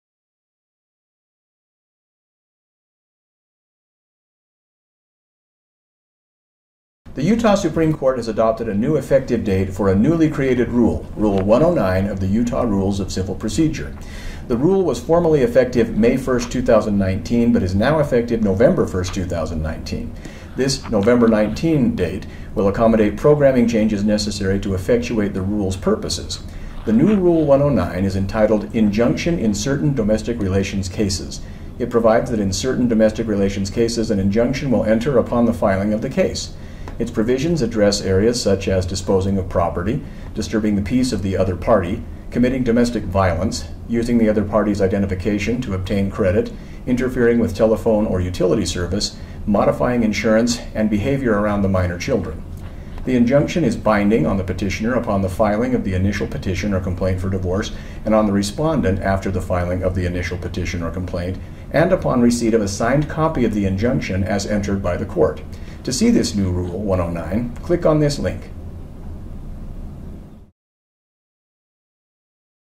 New Rule 109: [automatic] Injunction.
Find out from this video what Rule 109 does.

Utah Rules of Civil Procedure Rule 109: “[automatic] Injunction in certain domestic relations cases.”